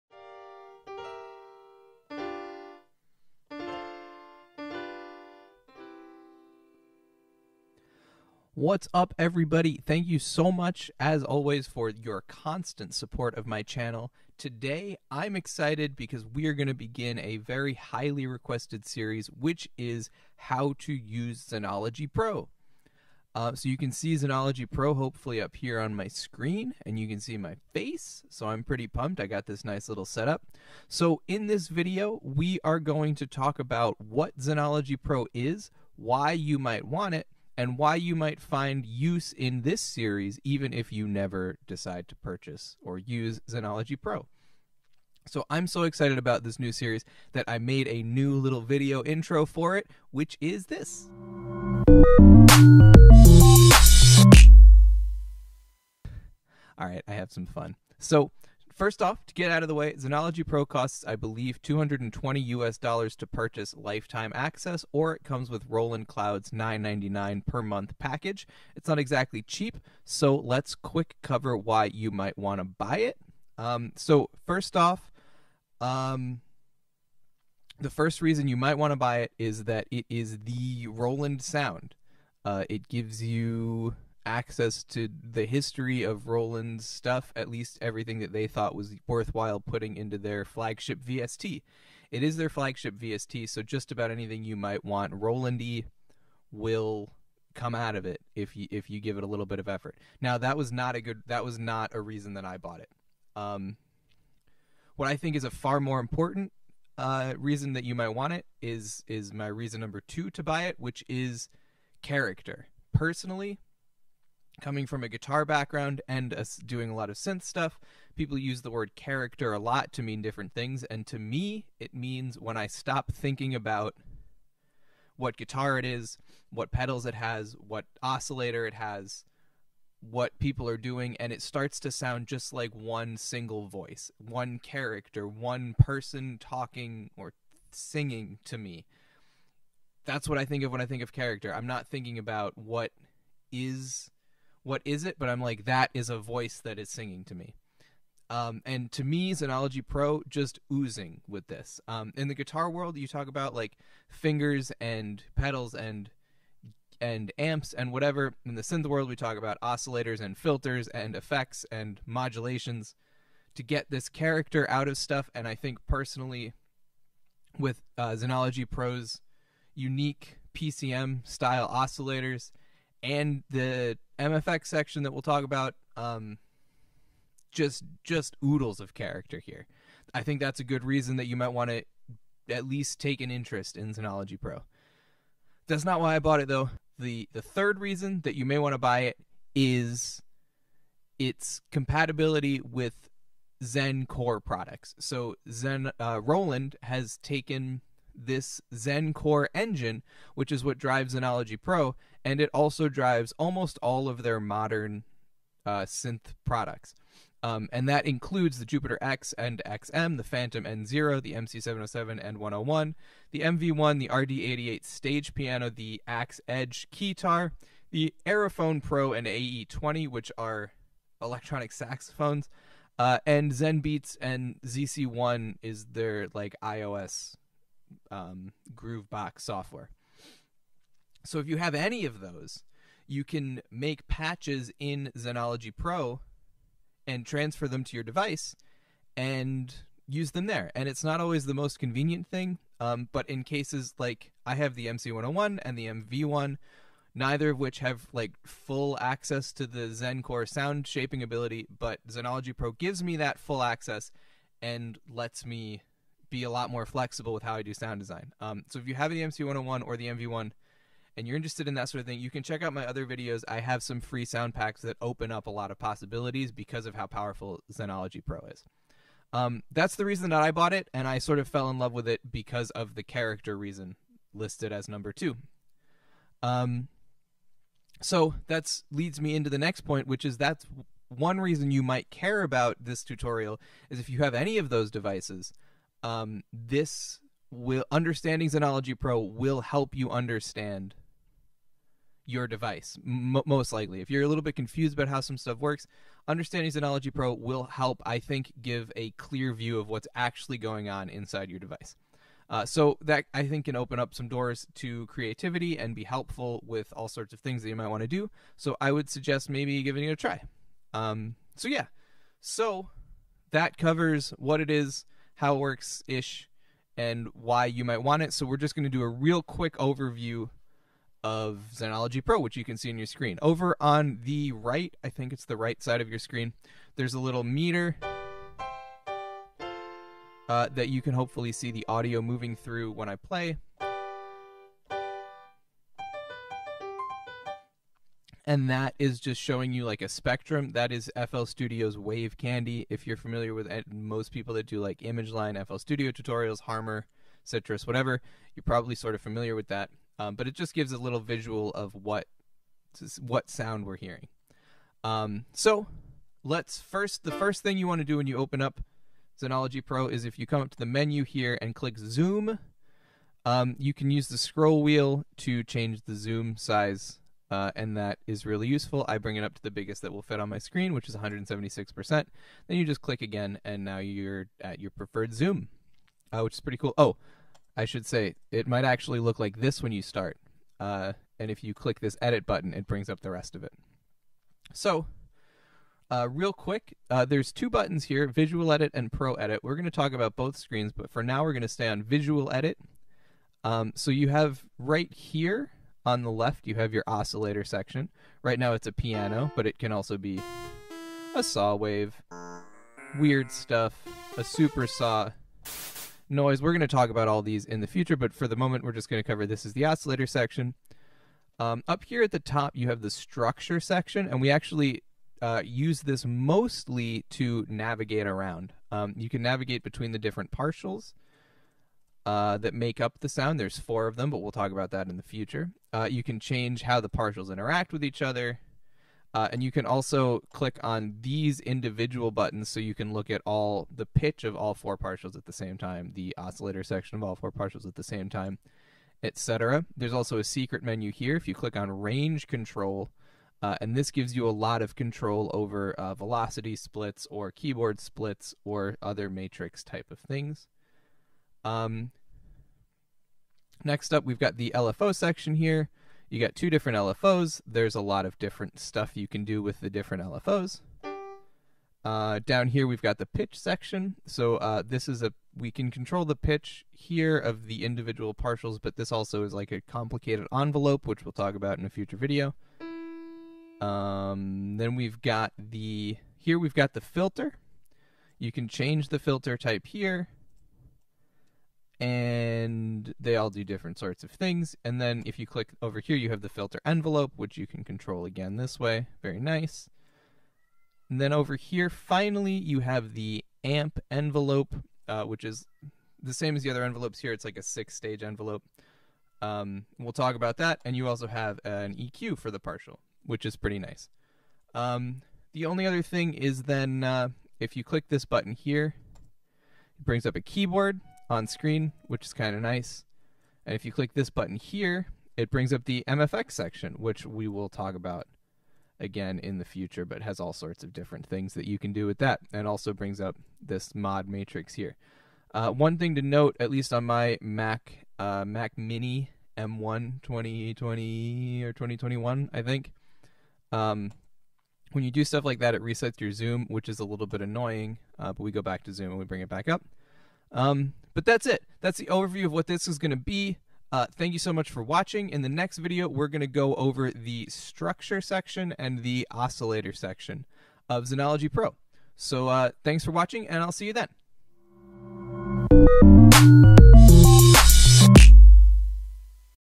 Zenology Pro for Beginners Lesson 0: Overview
Hi all! This is the beginning of my series on Zenology Pro! I hope it's useful, you can find the next video of the series here, don't hesitate to ask any questions I'll do my best to answer all of them!

Chapters:
0:00 Intro
1:00 Why Zenology Pro?
5:30 Zenology Pro Overview